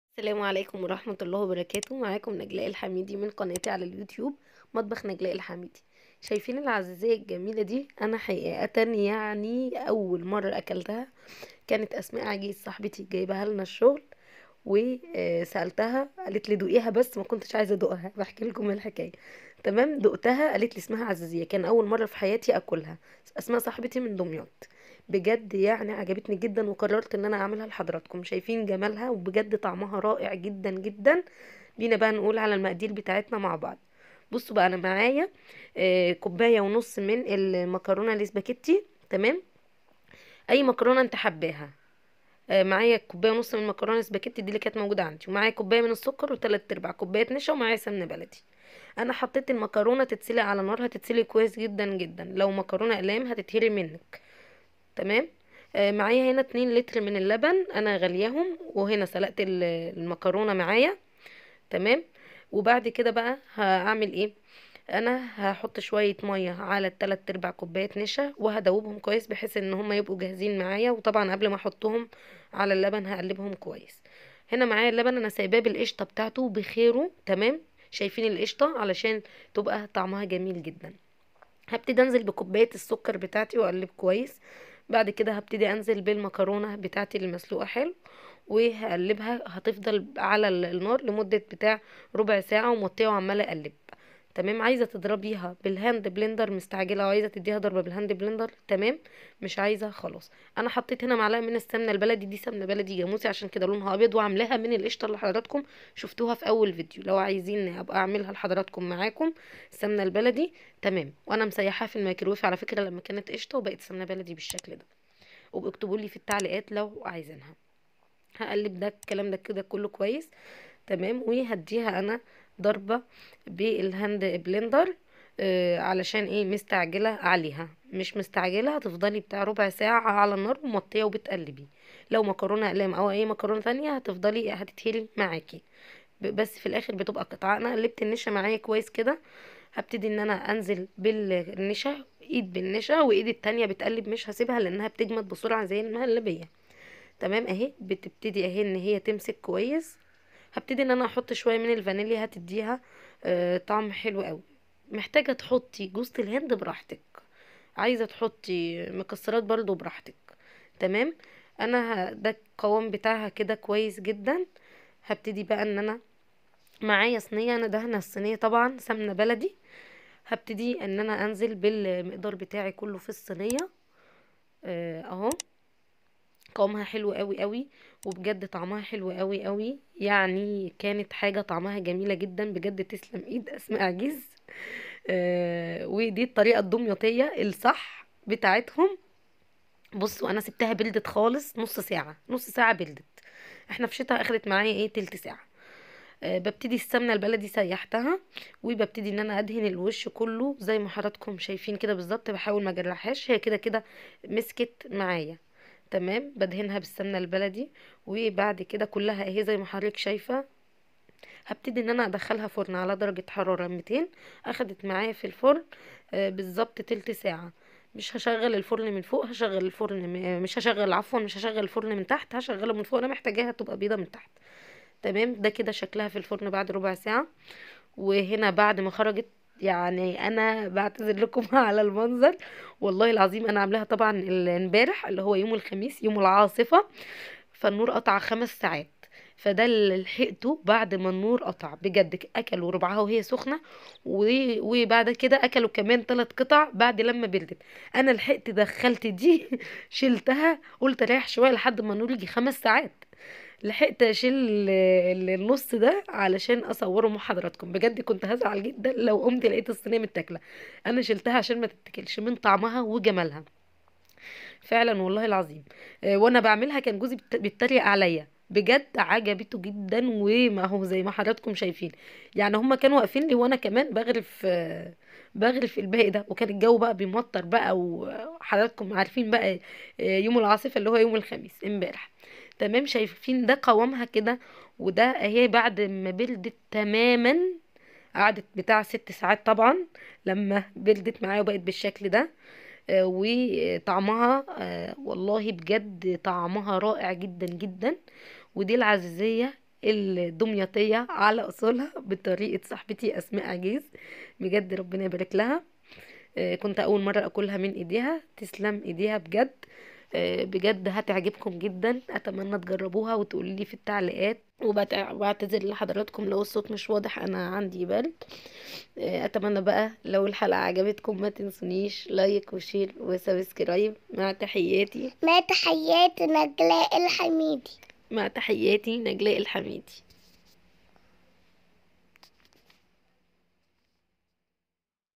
العزيزيه او المكرونه باللبن الدمياطيه على اصولها بجد طعمها فوق الوصف بطريقة صديقتى الدمياطيه
2كيلو حليب
كوب سكر
كوب ونصف مكرونه اسباجتى
معلقة سمنه بلدى
3/4 كوب نشا
فانيليا
الطريقه
يتم غلى الحليب  بعد ذلك يتم اضافة السكر  ثم نسلق المعكرونه ويتم اضافتها على الحليب ويضاف معلقة سمنه بلدى ثم تقلب  كل المكونات على نار هائه لمدة ربع ساعه ثم يضاف الفانيليا والنشا المذاب فى ماء ساقع وتقلب
وتدهن صينيه بالسمنه البلدى وننزل بالمكونات وتبرد تماما وتدهن بالسمنه البدى وتدخل فرن حرارته 200 لمدة تلت ساعه